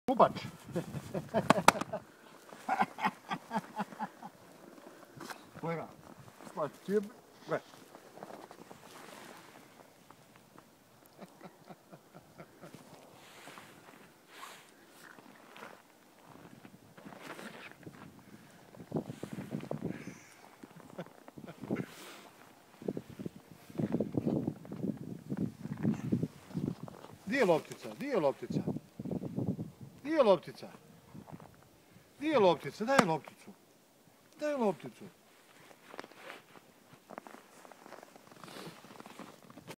Alaskan malamute playing fetch
Snoopy, 3 months old alaskan malamute playing fetch in snow.